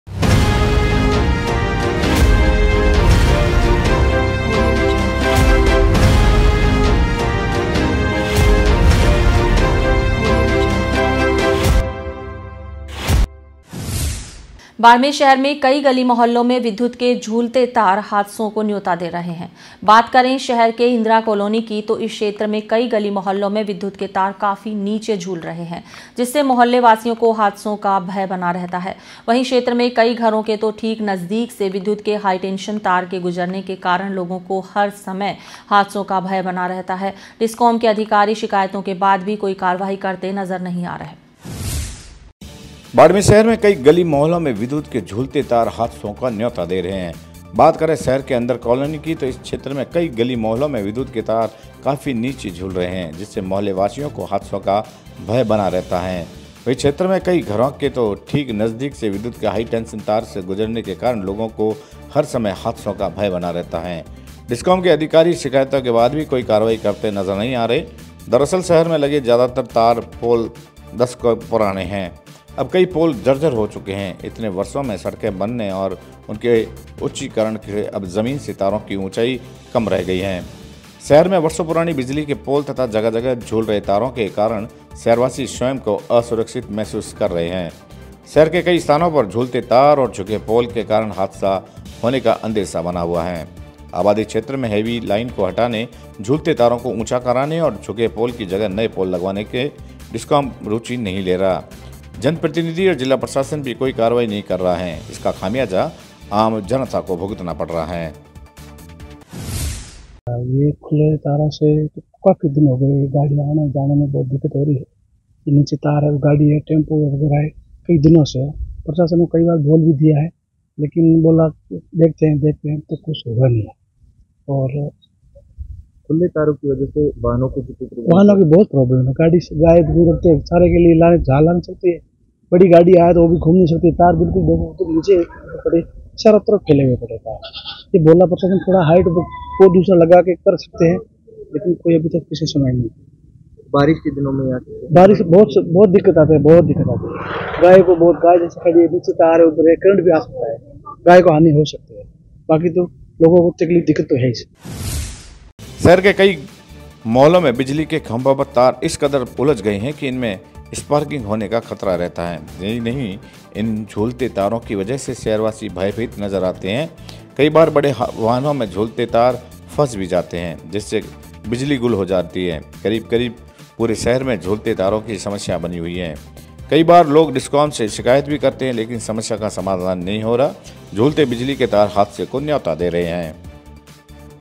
थारनगरी में झूलते तार बने जी का जंजाल, शहर में झूलते बिजली के तार दे रहे हादसे को निमंत्रण
थारनगरी में झूलते तार बने जी का जंजाल, शहर के विभिन्न गली-मोहल्लों में झूलते बिजली के तार दे रहे हादसे को निमंत्रण